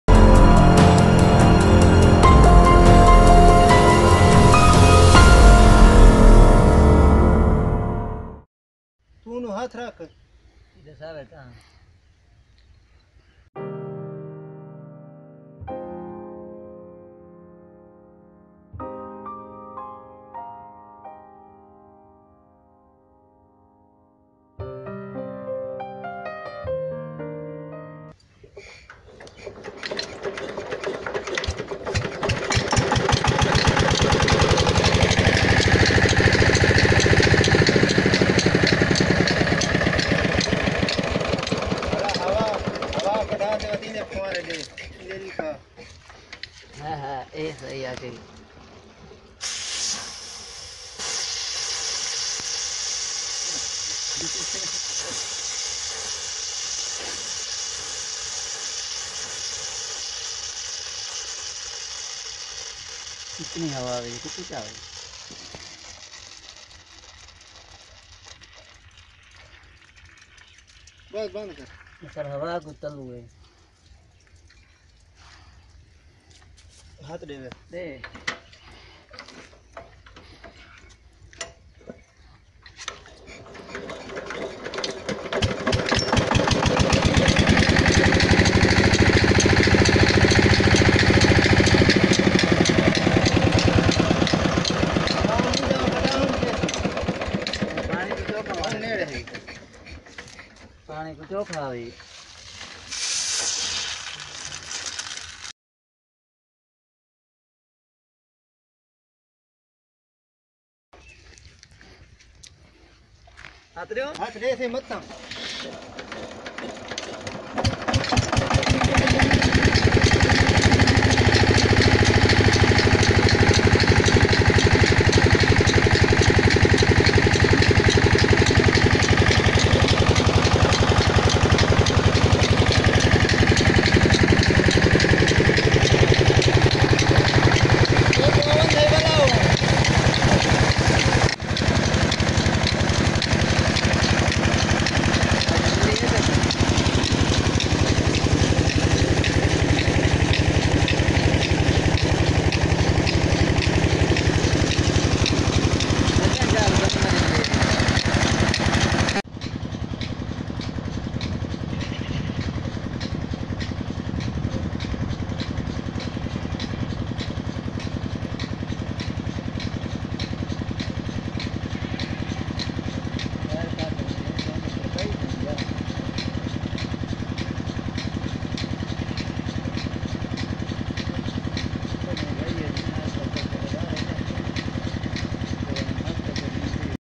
23 hp peter diesel engine working on water pump||Our punjab
.Welcome to our Channel "our punjab" is vedio men ham aap ko dekhaein gy 23 hp deisel engne cheef compny  k engine ki karkrdgi,aysi mzeed vedio dekhny k lye hmary channl ko subscribe zror krein ,thanx